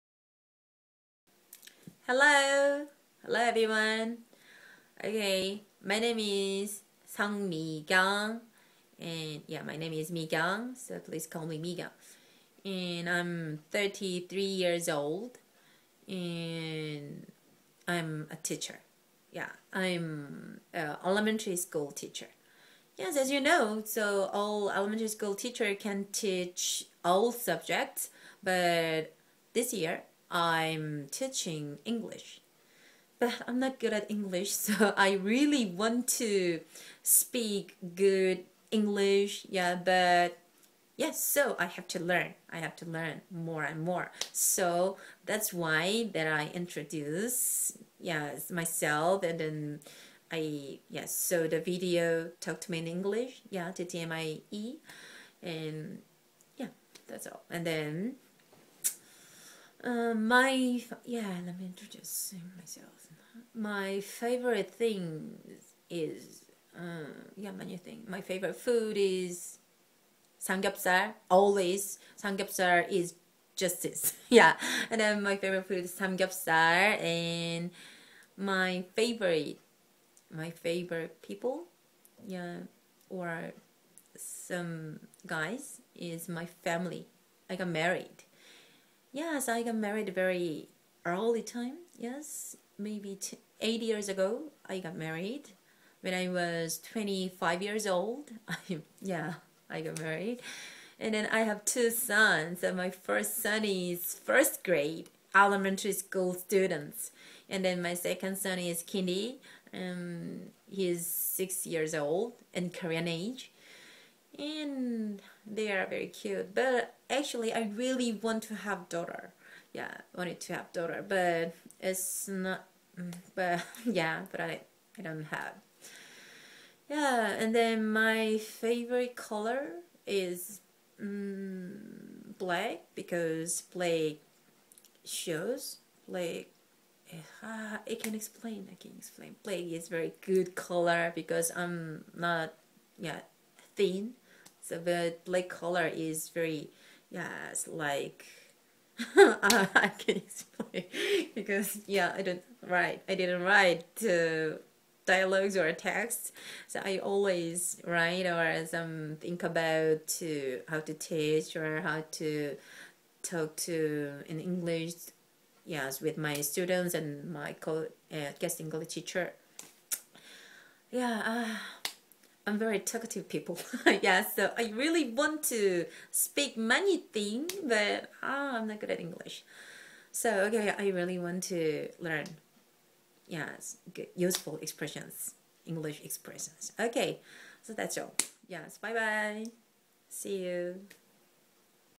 ttmie_introducing contest_Mikyung
I really want to join with you and learn English more. This video is my first, so I'm shy. :p